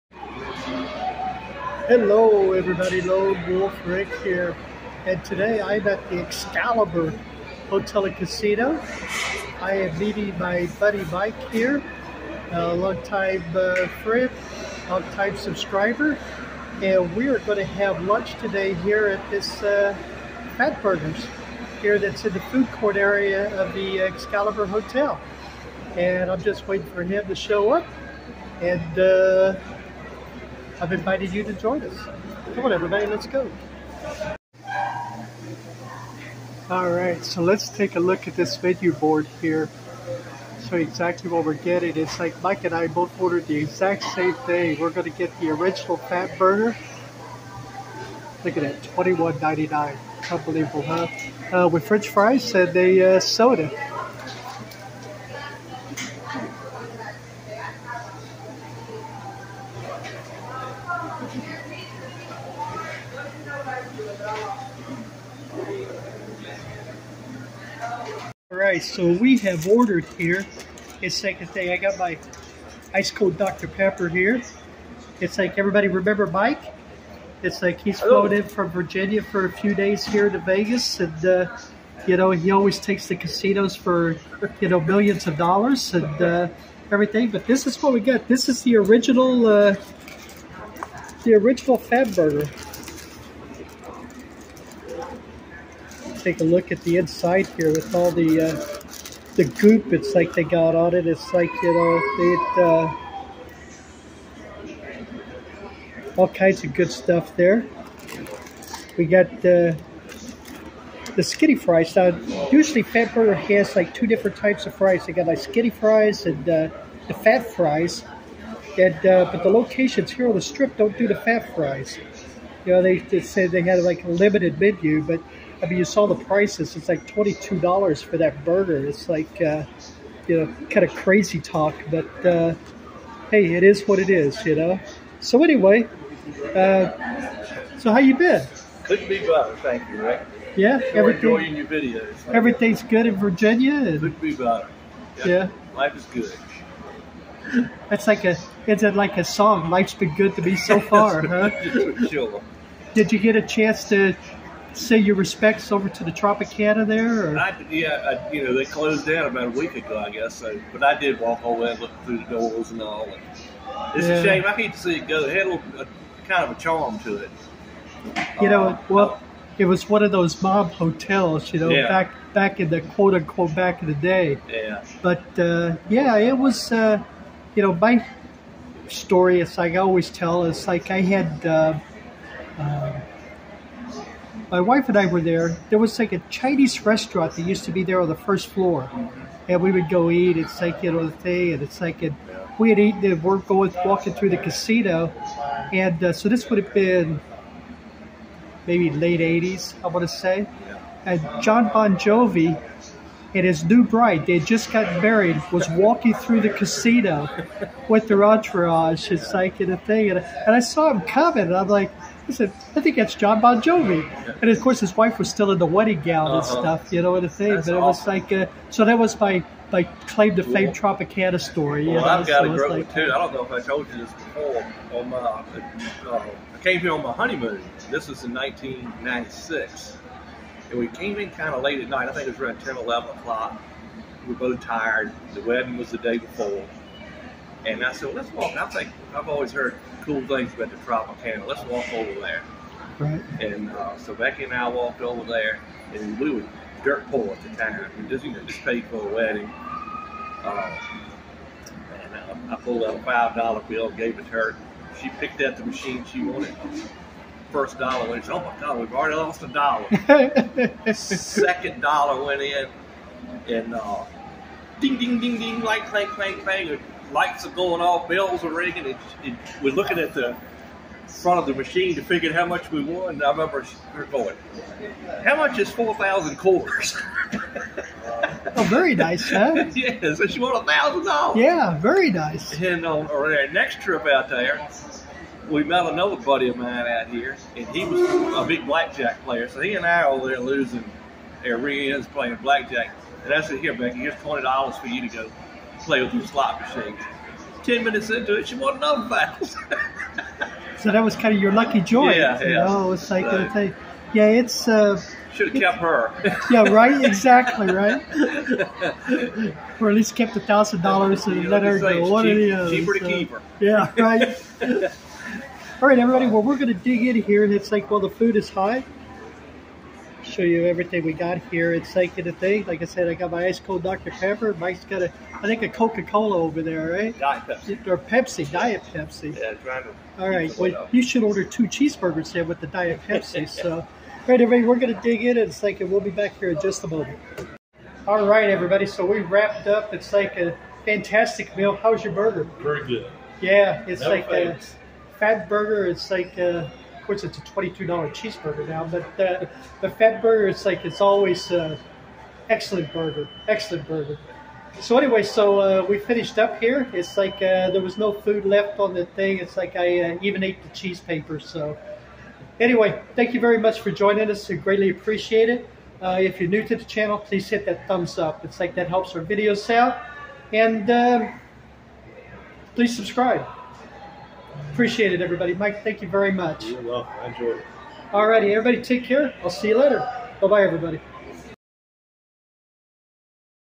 Lunch In Las Vegas. Meeting My Buddy Mike At FatBurger At The Excalibur Hotel.
Check out the memberships on my channel. Hit the JOIN button to read all about it.